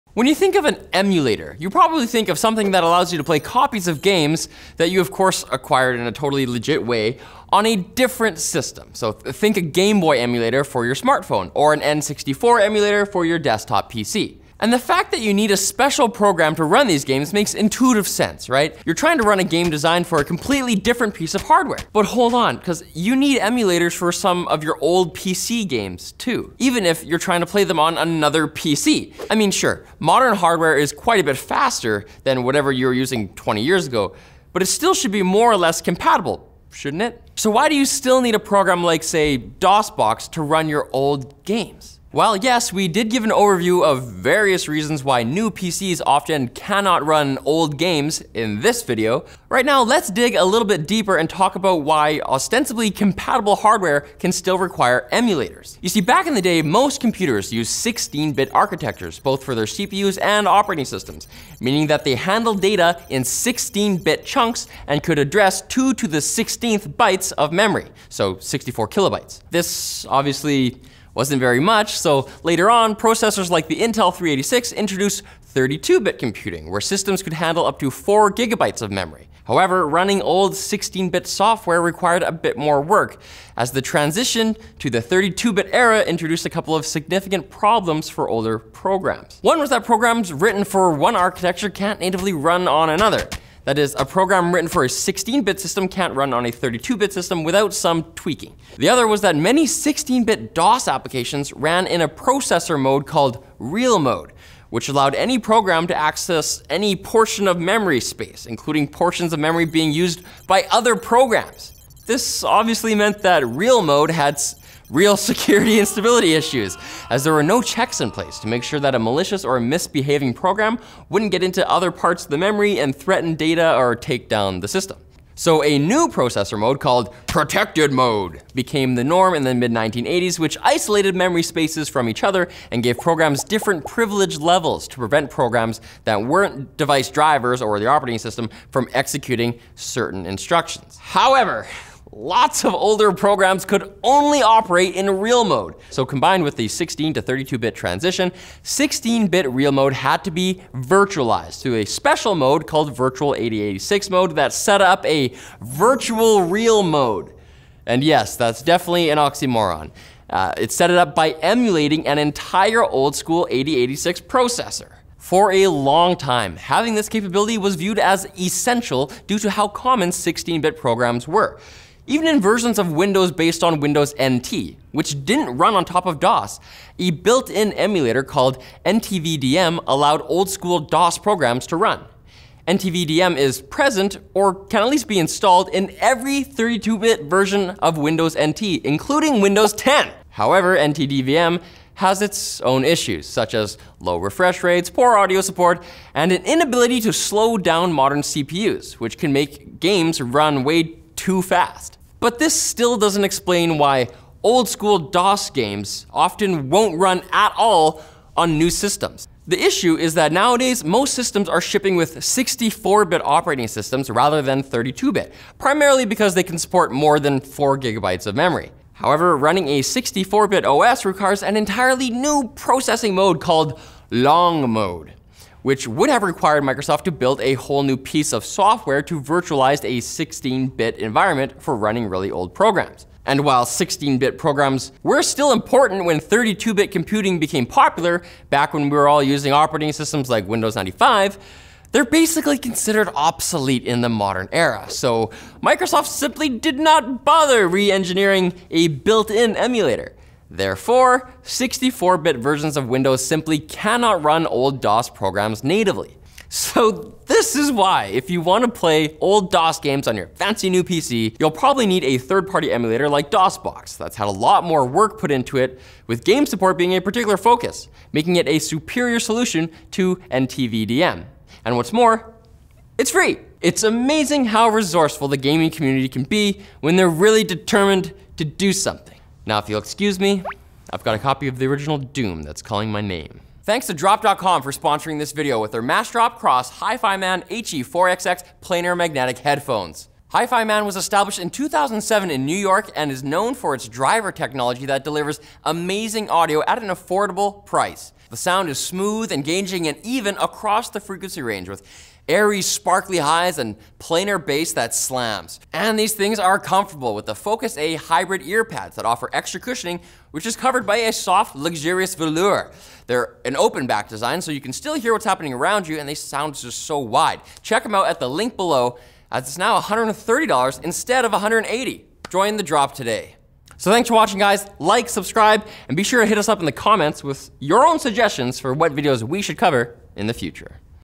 Why Do You Need DOSBox?
Check out the Massdrop x HIFIMAN HE4XX Planar Magnetic Headphones

Why do you need a PC emulator program to run games on...a PC?